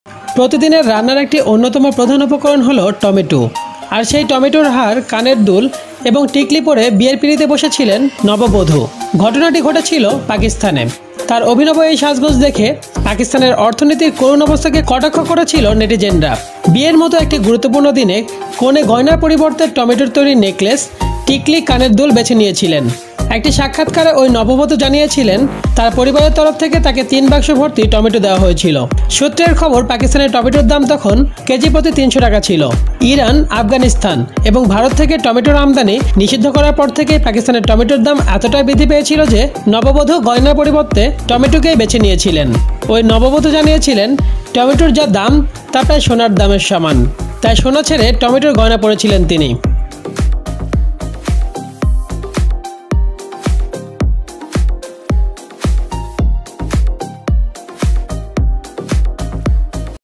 কেনো বিয়েতে টমেটোর গয়না পড়েছিলেন পাকিস্তানি নববধূ ! Why Pakistani Bride Wore Tomato Jewellery?
ˍˍˍˍˍˍˍˍˍˍˍˍˍˍˍˍˍˍˍˍˍˍˍˍˍˍˍˍˍˍˍˍˍˍˍˍˍˍˍˍˍˍˍˍˍˍˍˍˍˍˍˍˍˍˍˍˍˍˍˍˍˍˍ